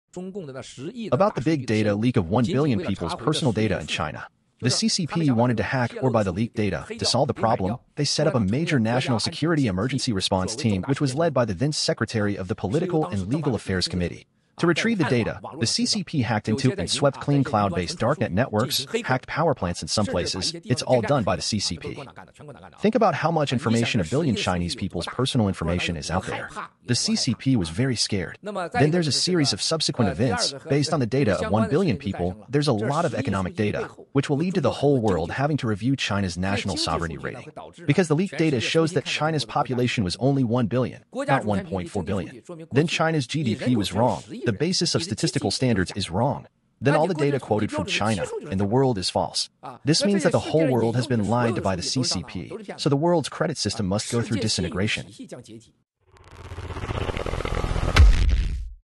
In July 2022, the media reported that the personal data of 1 billion Chinese people in China had...
In July 2022, the media reported that the personal data of 1 billion Chinese people in China had been leaked.
Miles Guo revealed that in order to retrieve the data, the Chinese Communist Party set up a major national security emergency response team, led by the then secretary of the Political and Legal Affairs Commission, which swept cleaned and hacked many cloud storage darknets and power plants around the world.